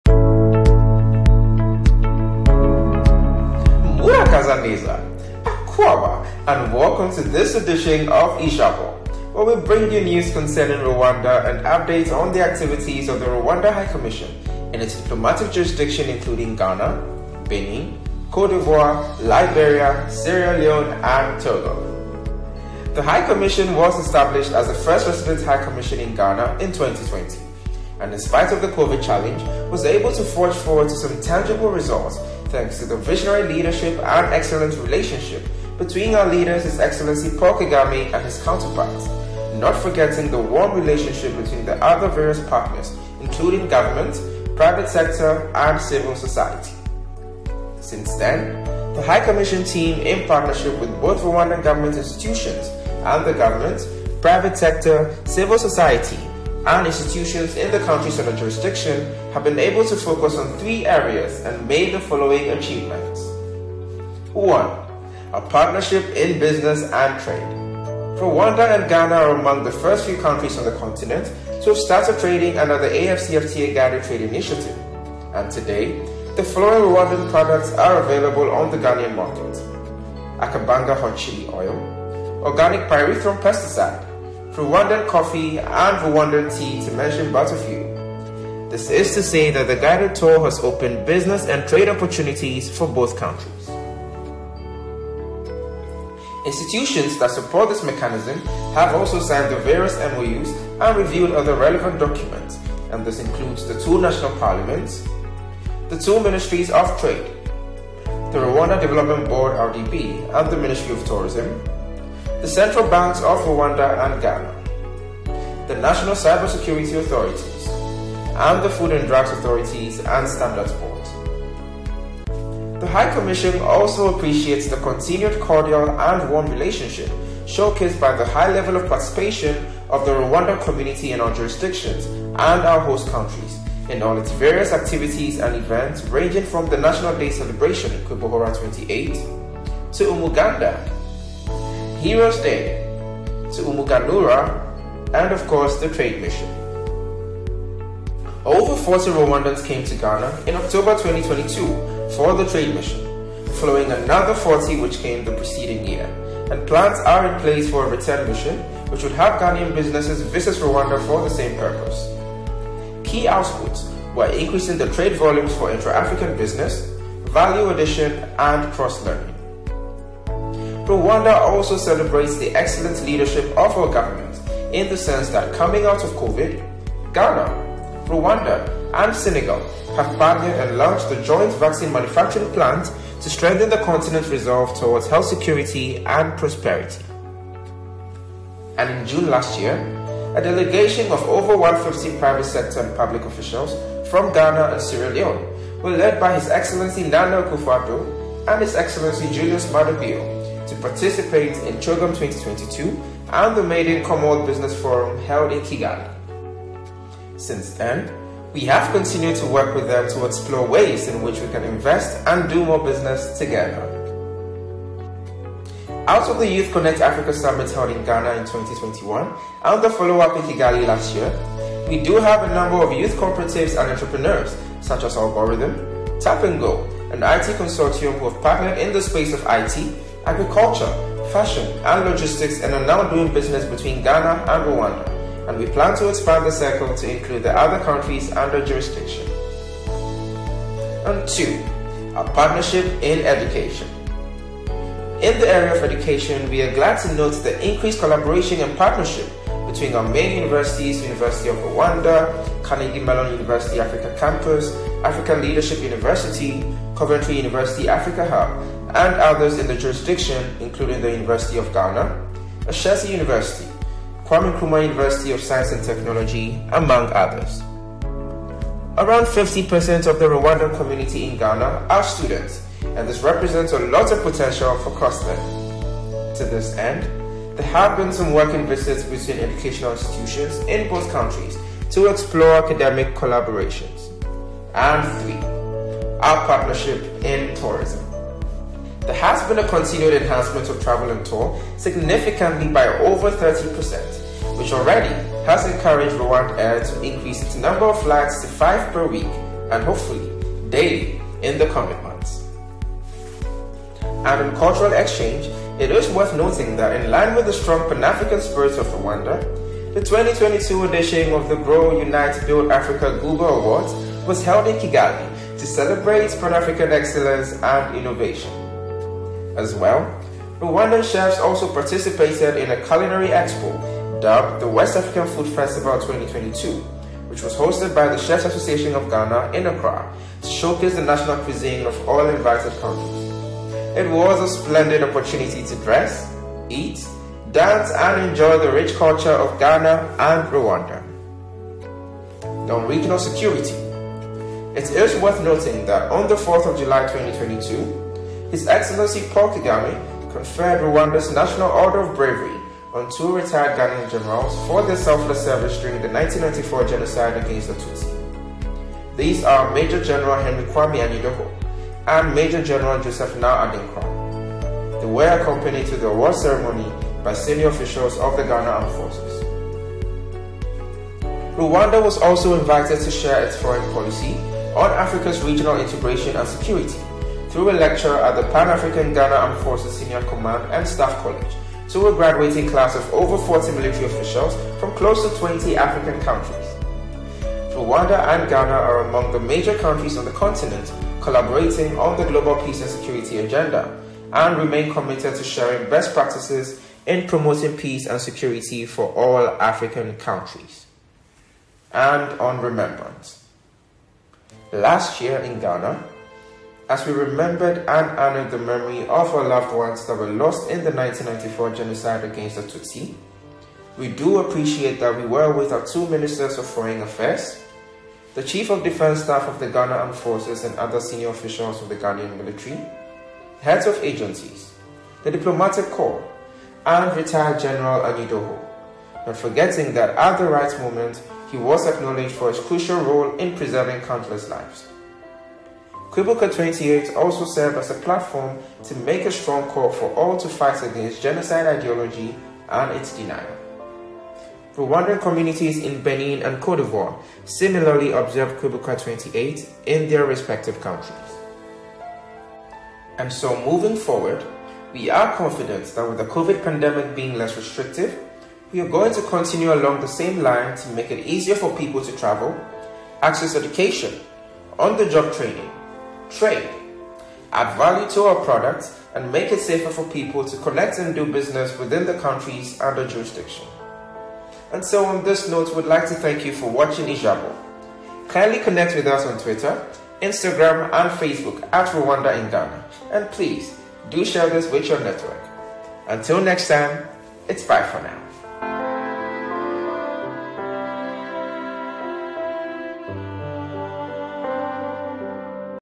Ijabo Episode 1.
As we begin 2023, we are delighted to share with you some of the key highlights and milestones achieved in 2021/22.

Our sincere gratitude to Our Leadership and Partners for your invaluable contribution to this journey. We look forward to stronger cooperation between 🇷🇼 & 🇬🇭, 🇨🇮, 🇧🇯, 🇹🇬, 🇱🇷 & 🇸🇱, and enhanced collaboration with all our partners this year & beyond!

Murakoze cyane!